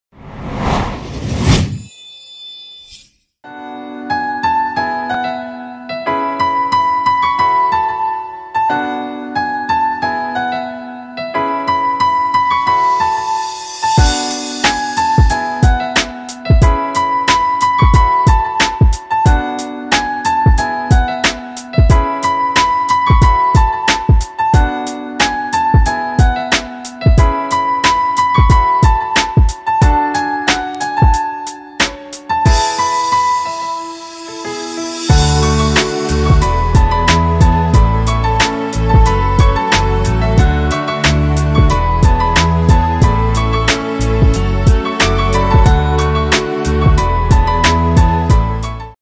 Geniqua Recliner Lounge Chair Black Leather Overstuffed Pad Cushion
Our new black leather manual push-back recliner chair. Featuring soft comfortable upholstered sofa which can provides you fully rest while reading or watching TV